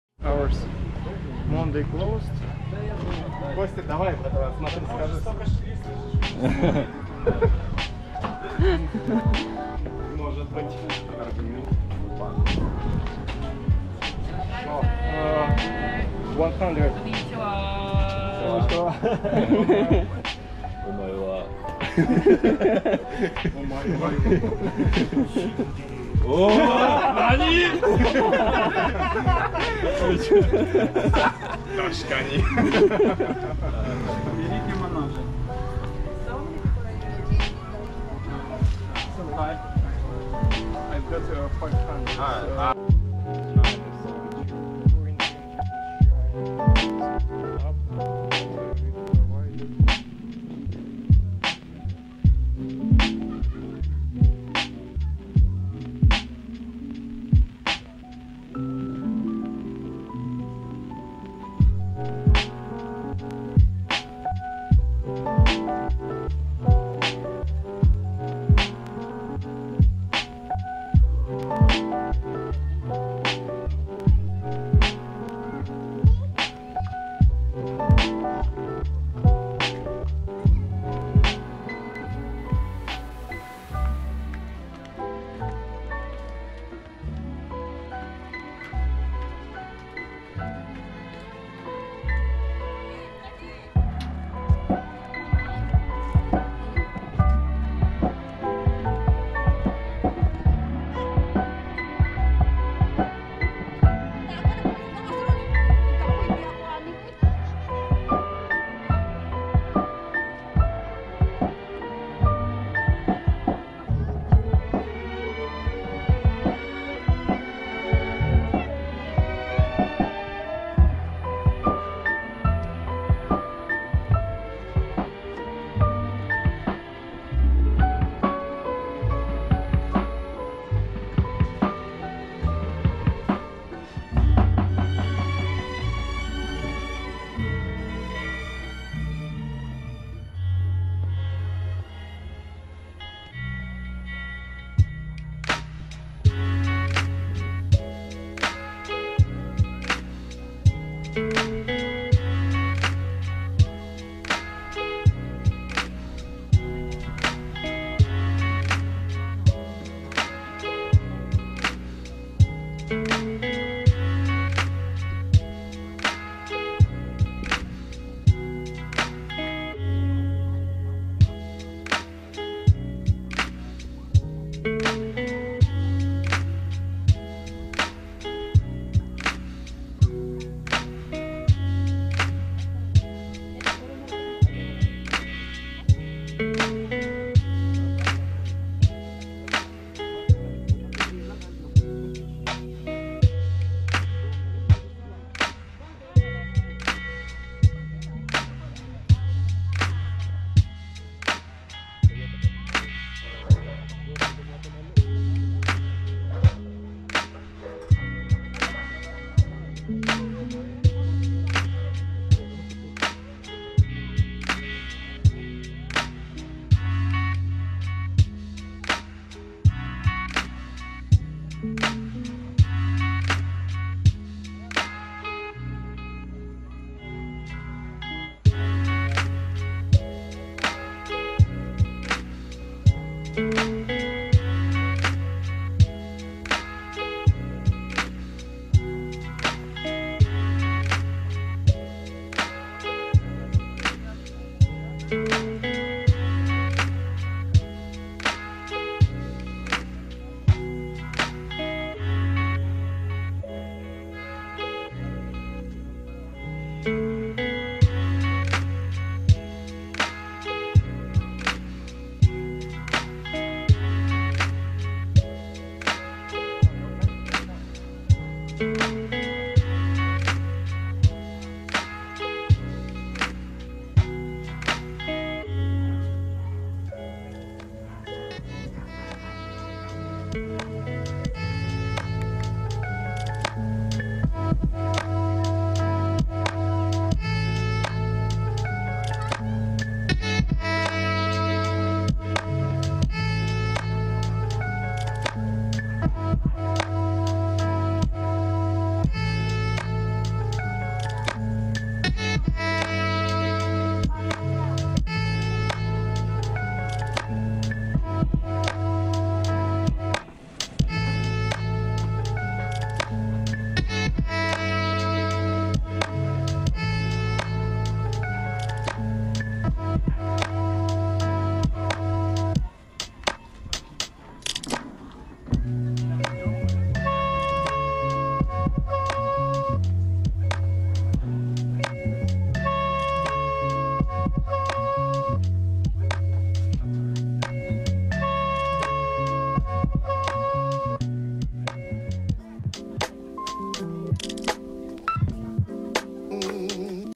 Sachiko's Little Kyoto, кусочек Японии на одном из островов Филиппин, под легкий Lo-fi hip-hop
Это завершающее место одного дня, кусок Японии где-то далеко в горной местности острова Себу, часть 3.